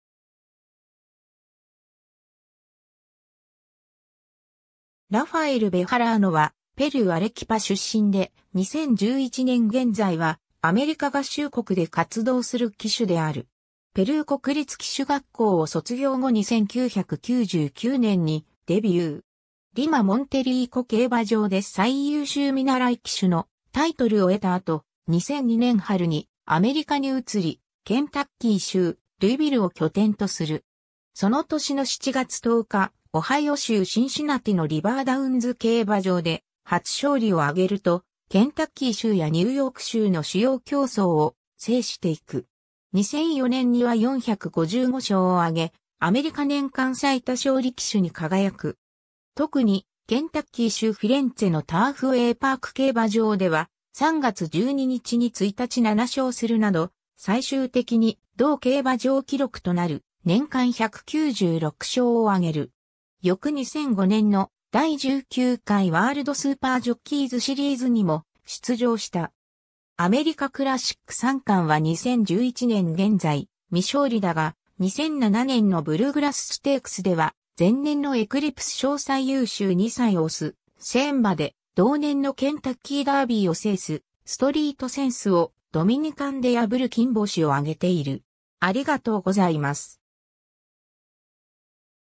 ラファエル・ベハラーノ
ラファエル・ベハラーノ（Rafael Bejarano、1982年6月23日 - ）は、ペルー・アレキパ出身で、2011年現在はアメリカ合衆国で活動する騎手である。
 ペルー国立騎手学校を卒業後に1999年にデビュー。
リマ・モンテリーコ競馬場で最優秀見習騎手のタイトルを得た後、2002年春にアメリカに移り、ケンタッキー州ルイビルを拠点とする。
その年の7月10日、オハイオ州シンシナティのリバーダウンズ競馬場で初勝利を挙げると、ケンタッキー州やニューヨーク州（ニューヨーク競馬協会）の主要競走を制していく。
 2004年には455勝を挙げ、アメリカ年間最多勝利騎手に輝く。
特にケンタッキー州フィレンツェのターフウェイパーク競馬場では、3月12日に1日7勝するなど、最終的に同競馬場記録となる年間196勝を挙げる。
翌2005年の第19回ワールドスーパージョッキーズシリーズにも出場した。
 アメリカクラシック三冠は2011年現在未勝利だが、2007年のブルーグラスステークスでは、前年のエクリプス賞最優秀2歳牡・セン馬で同年のケンタッキーダービーを制すストリートセンスをドミニカンで破る金星を挙げている。